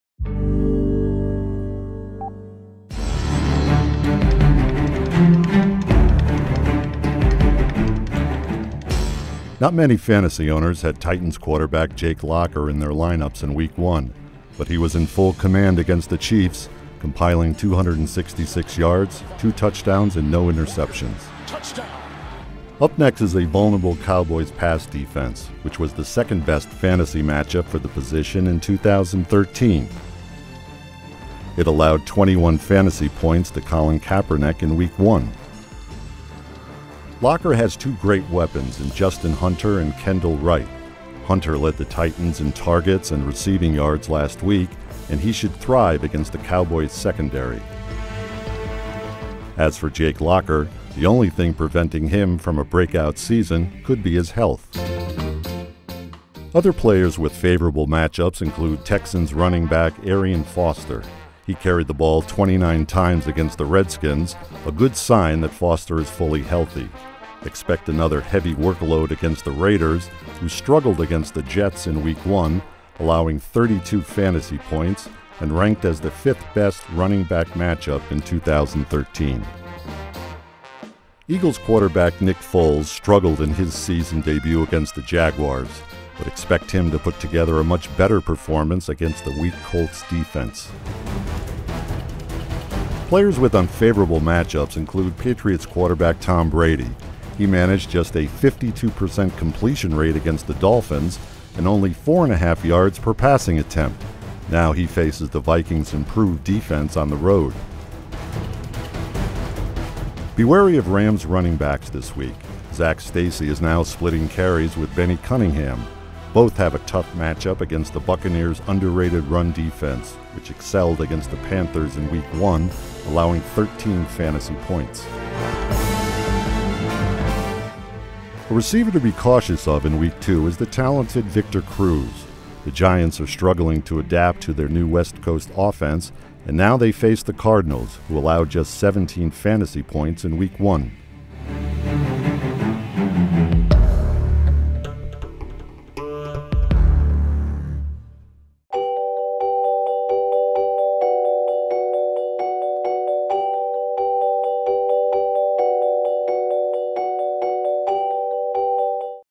Fantasy Football 2014: Picks for Week 2 | The New York Times
In a new weekly video series about fantasy football, The Times assesses the fantasy points allowed per game for various positions. For Week 2, players may want to look at quarterbacks.

Produced by: Justin Sablich and Vijai Singh

Fantasy Football: Picks for Week 2 | The New York Times